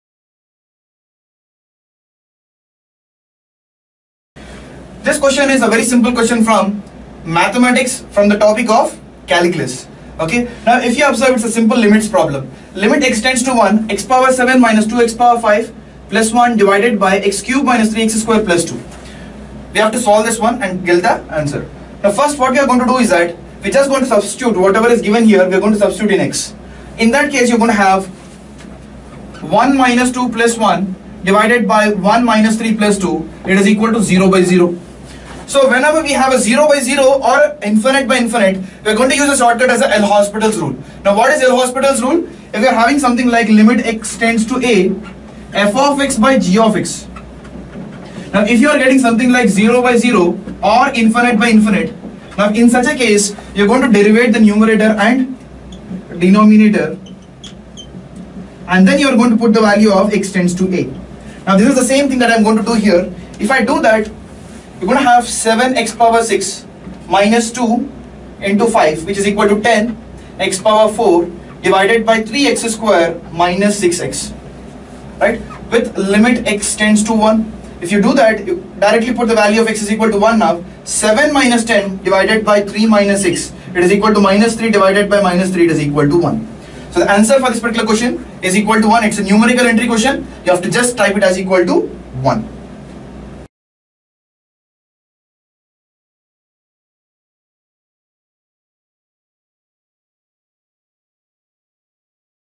GATE 2017 Solutions | CS | 11 - Feb Morning Session | Mathematics
The video  explains GATE 2017 Question & Solution. For GATE 2017 complete Papers with Solutions visit this link:  goo.gl/CF9LXY

Correspondence Course- Comprises of hard-copy GATE focused books options of Tests and video lectures. Its popular amongst self-preparing GATE aspirants.